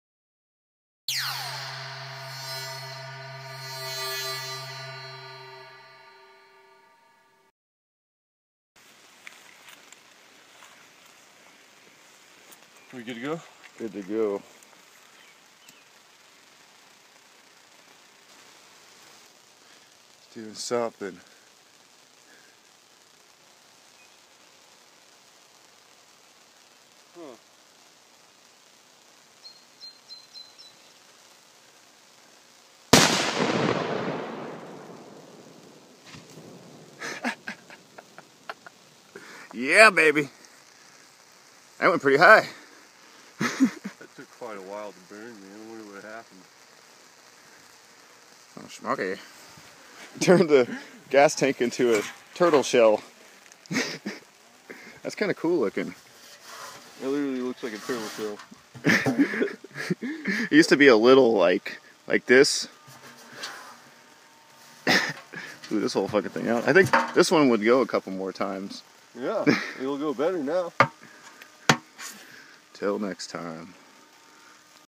Gas tank goes BOOM! :-D
I got a real big firecracker :-D it's for science ;-)